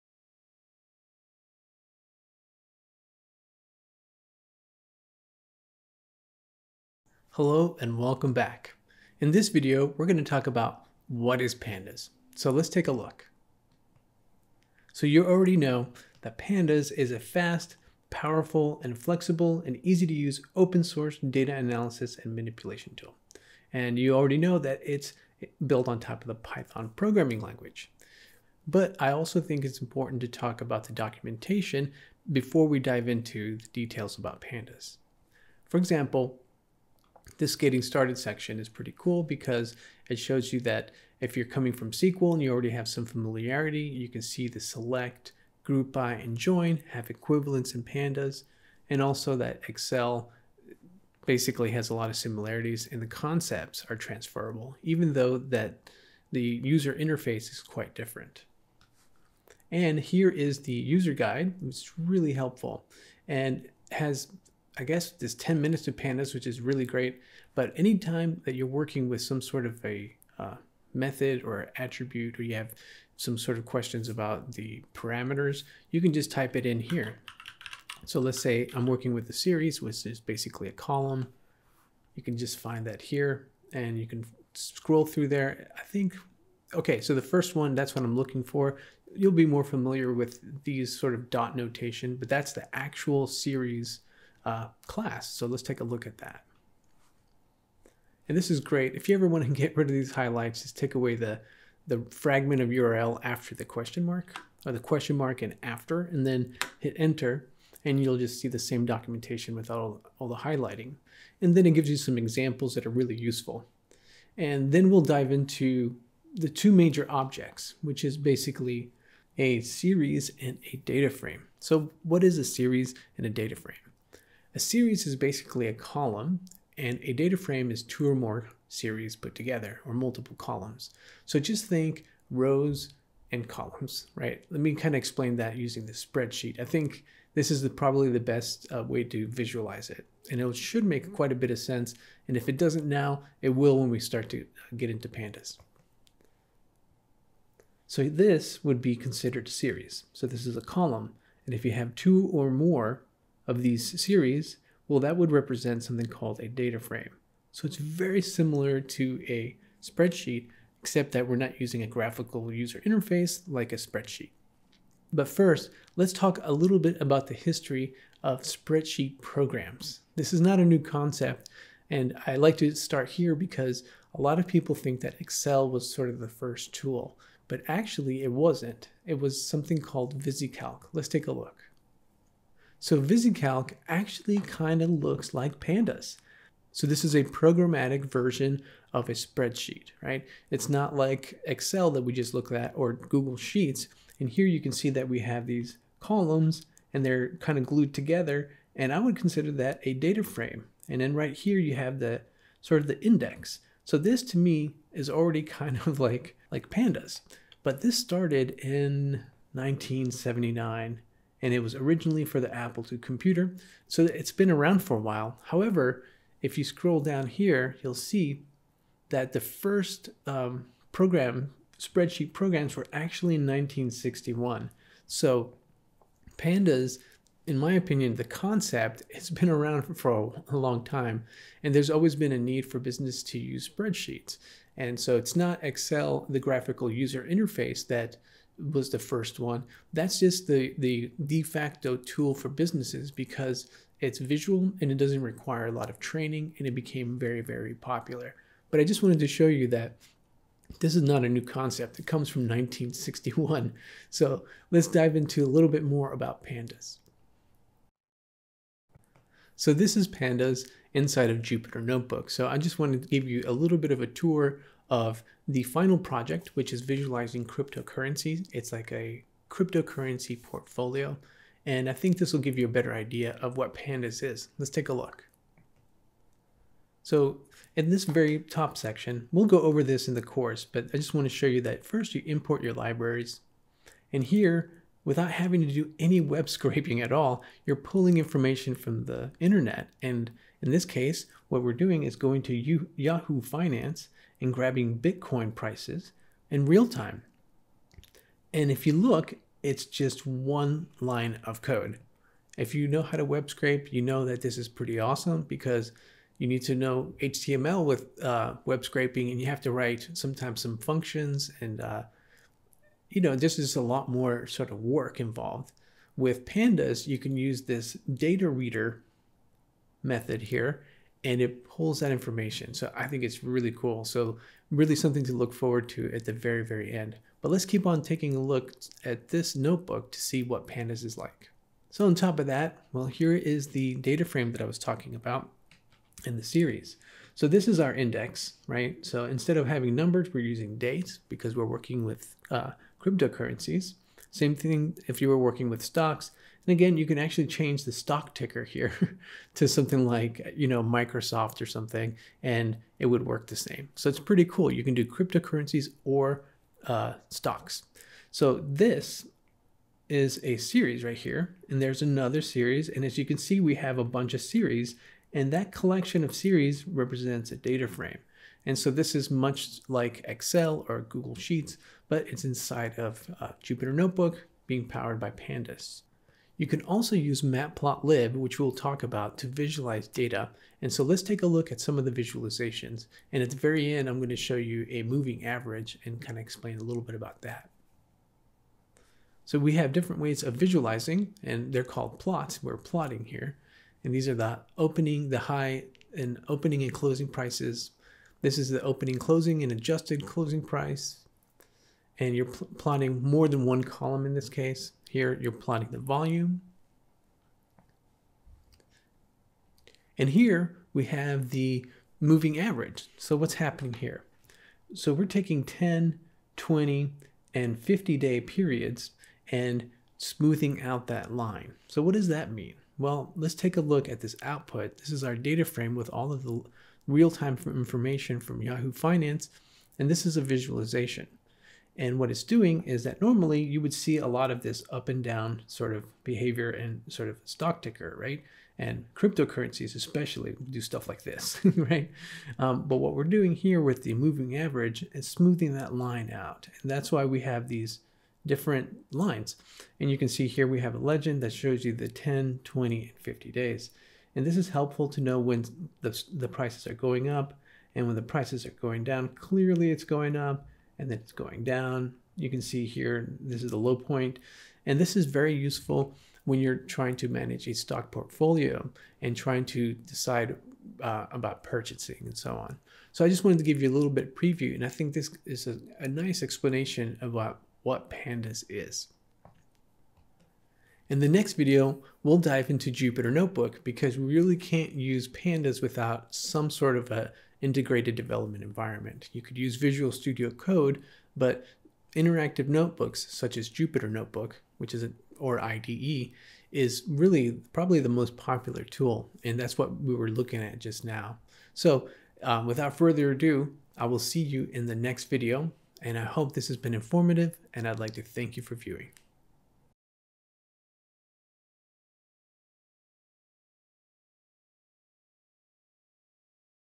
What is Pandas? | Python and pandas for Data Manipulation Training
CBT Nuggets Trainer Jonathan Barrios breaks down the popular open-source library, Pandas, using real life examples.

There’s a reason why many data scientists vote Pandas as the best data analysis and manipulation tool available. Pandas makes it simple to do many of the time-consuming, repetitive tasks associated with working with data. As one of the most popular data-wrangling packages, Pandas works well with many other data science modules inside the Python ecosystem. Keep on watching to learn more.